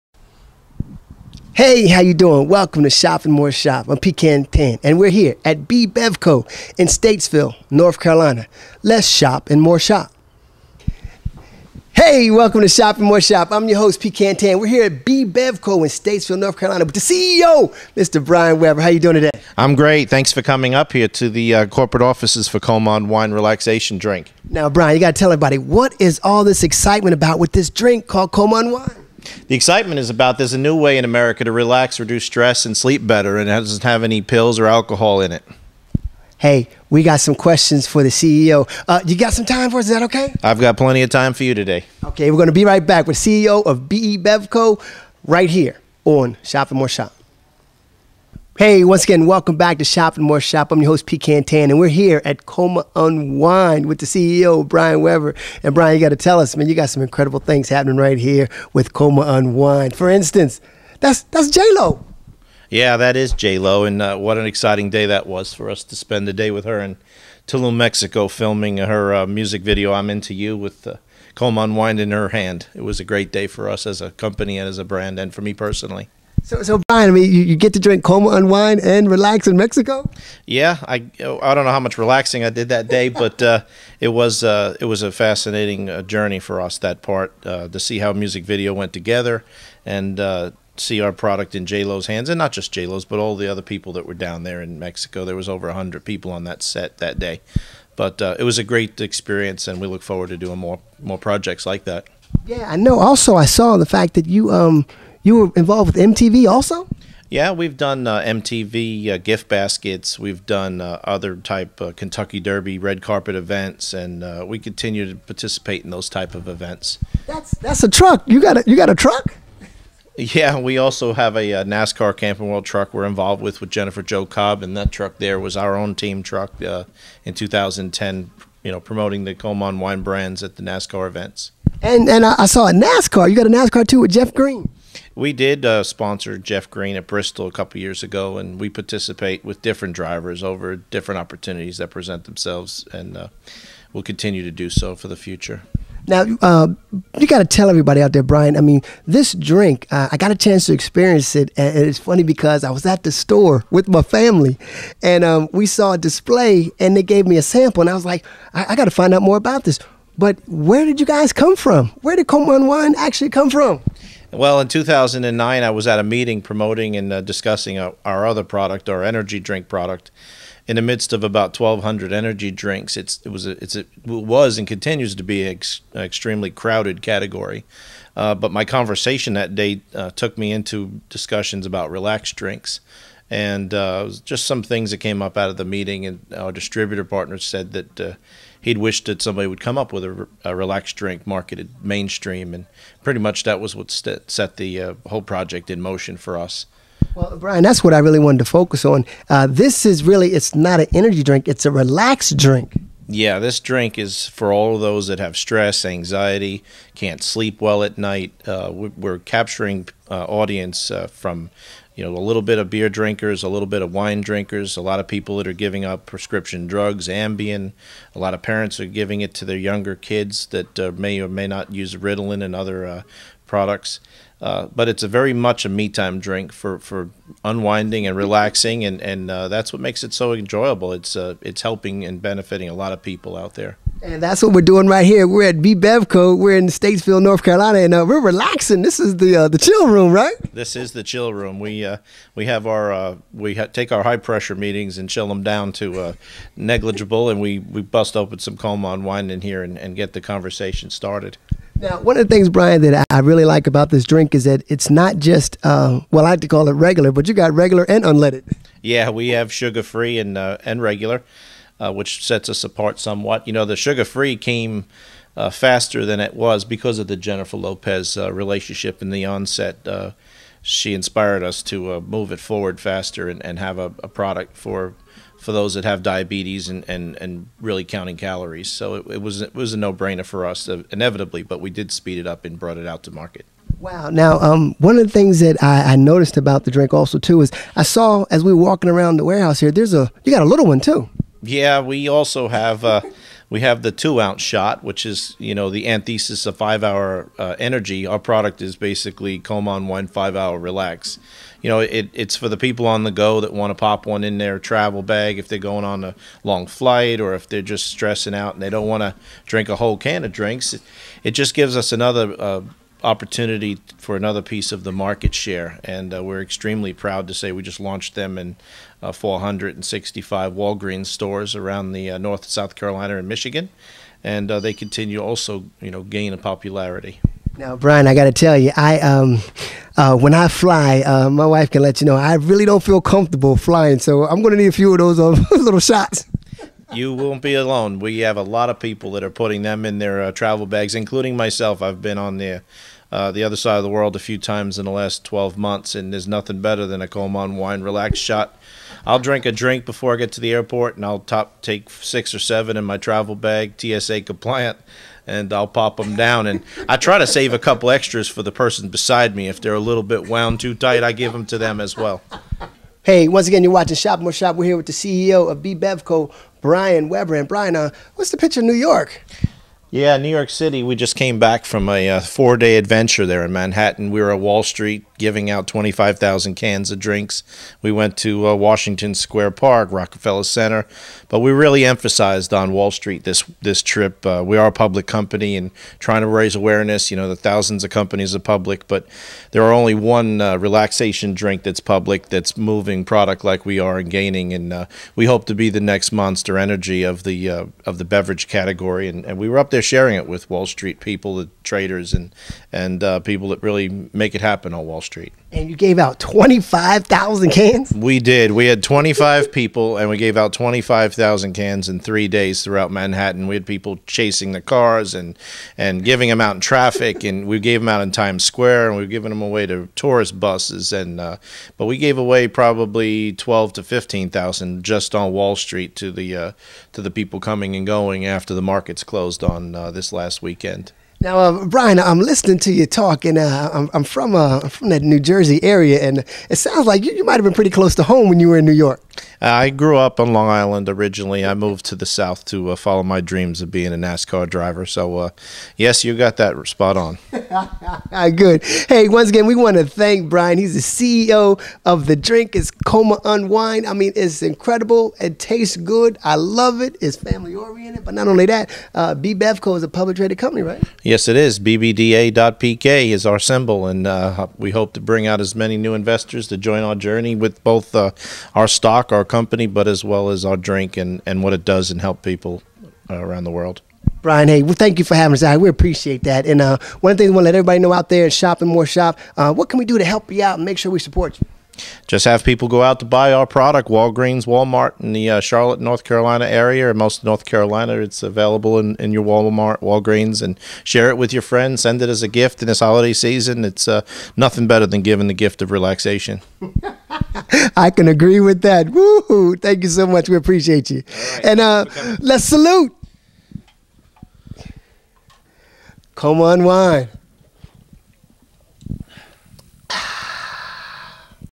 The KOMA Unwind Story!
KOMA Unwind relaxes those who drink it and helps them improve both the duration and quality of their sleep with flair and flavor which all are sure to enjoy. Koma Unwind contains a combination of Valerian Root, Milk Thistle, Rose Hips, B-12, and Melatonin. A Chillaxation Drink, Koma Unwind is formulated to relax the body, mind, and soul. It is the perfect lifestyle beverage for those who can appreciate extreme relaxation. So sip a Koma Unwind and Chill without a pill.